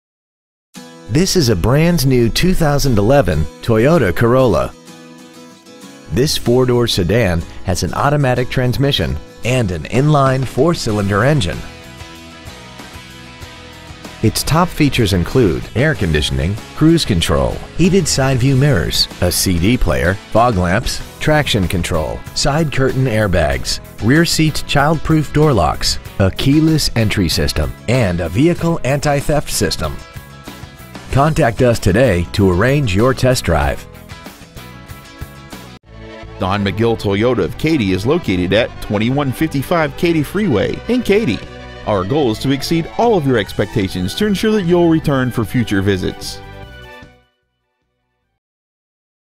2011 Toyota Corolla Katy TX
Year : 2011
 Make : Toyota
 Model : Corolla
 Engine : 4 Cyl.
 Trans . : Automatic
 Exterior : Super White

 Interior : Dark Charcoal
 Stock : K31059

 2011 Toyota Corolla Katy TX